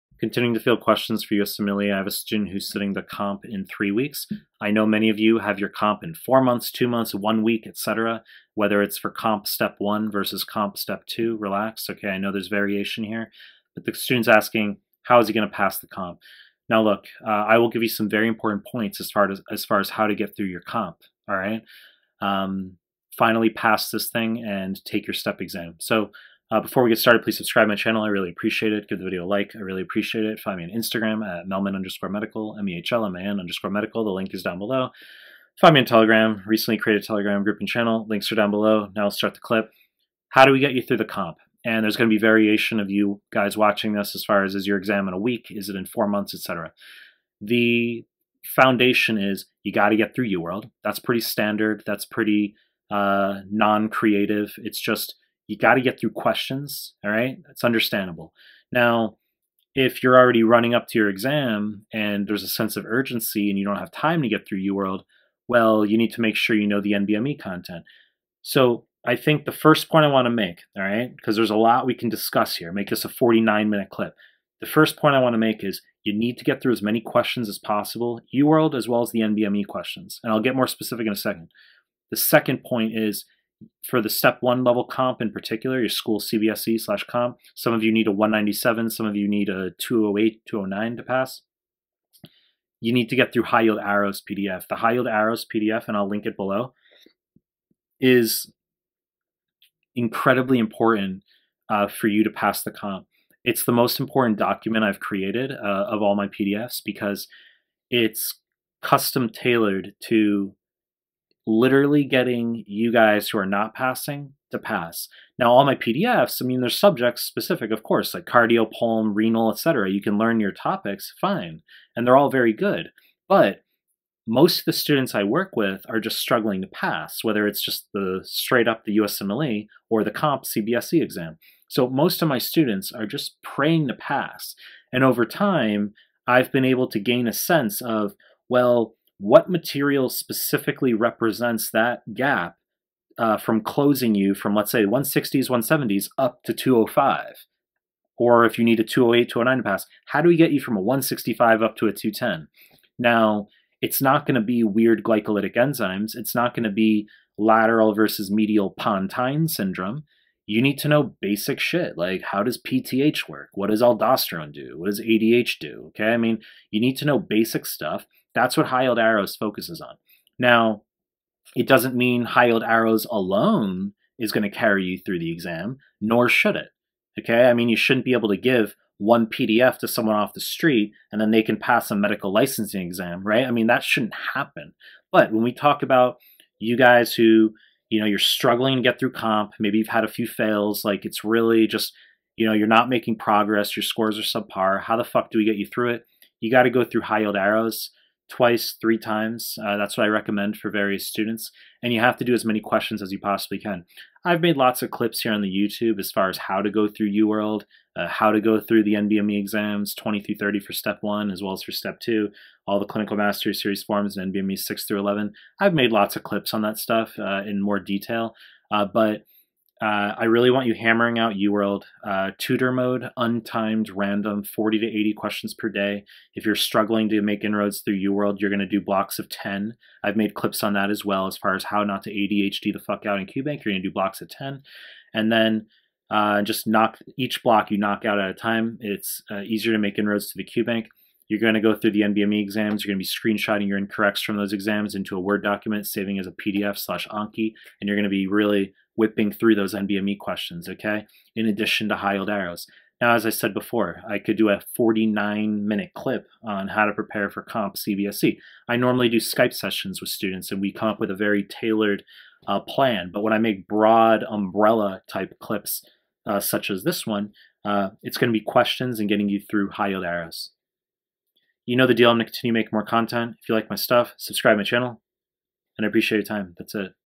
"My scores are low. How do I finally pass my school COMP / CBSE?"
In this clip I answer a student's question about how to finally pass CBSE / COMP, which some schools require prior to sitting the Step. I focus mainly on discussion of passing Step 1-level COMP/CBSE in this clip.